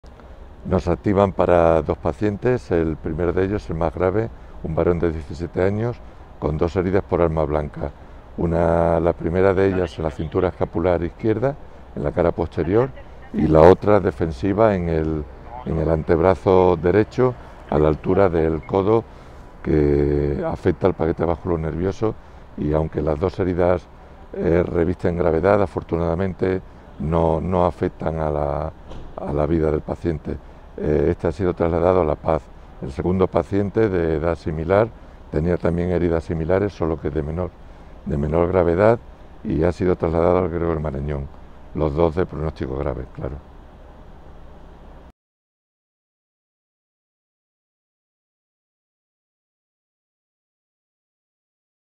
Dos jóvenes heridos por arma blanca en Ciudad Lineal 7agos2021
SAMUR-PC ha atendido a dos varones con varias heridas por arma blanca, tras una reyerta en Ciudad Lineal. Las dos víctimas han sido trasladadas, una vez estabilizadas, al hospital de La Paz y al Gregorio Marañón con pronóstico grave.
Policía Nacional se ha hecho cargo de la investigación de este suceso.